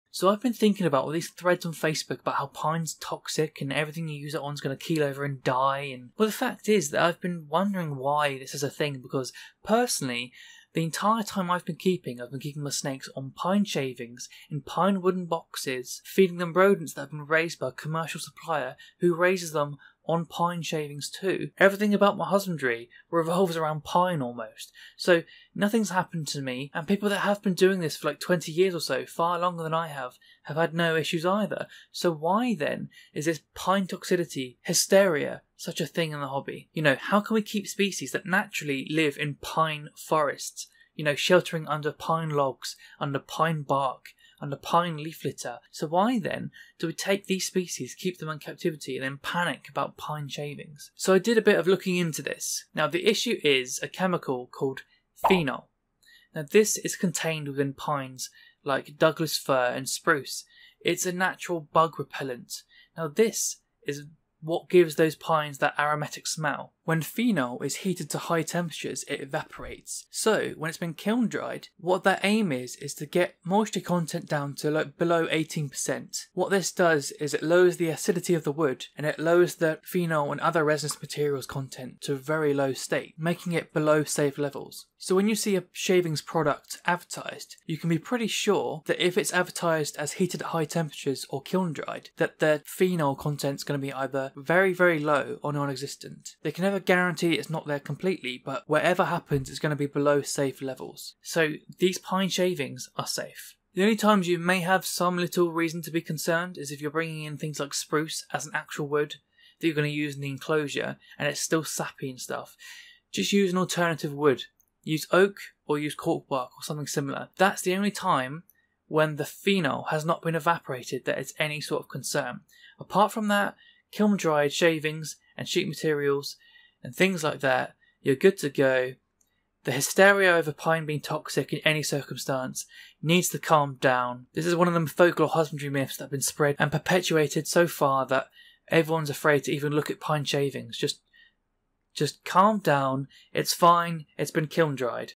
Is Pine Toxic To Bearded Dragons? Is Pine Bedding Safe for Reptiles?
Is pine toxic to reptiles? All I see on Facebook threads is stay away from pine its toxic to reptiles. So in this video we explore behind the idea that pine is so toxic it must be avoided at all costs.

Once kilm dried the toxic part of pine called phenol evaporates leaving pine shavings non toxic and safe for reptiles.